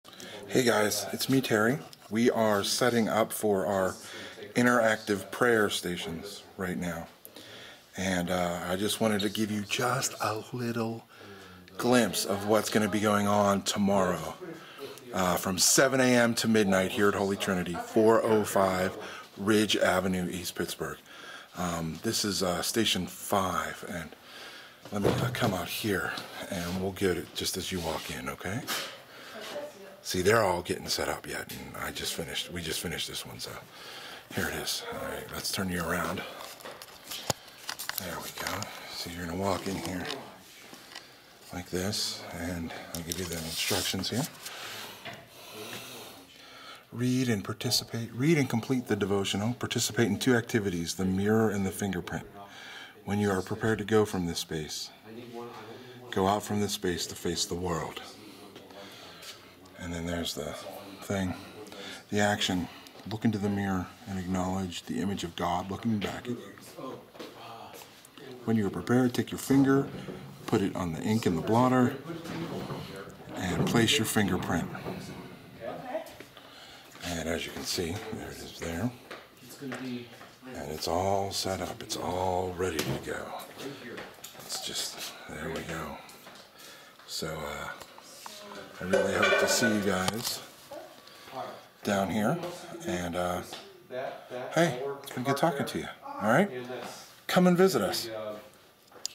Prayer Stations Good Friday at Holy Trinity
Holy Trinity Lutheran Church
Good friday March 25 2016
405 ridge ave
east pittsburgh
7am to Midnight

Come Join us for the 8 Prayer stations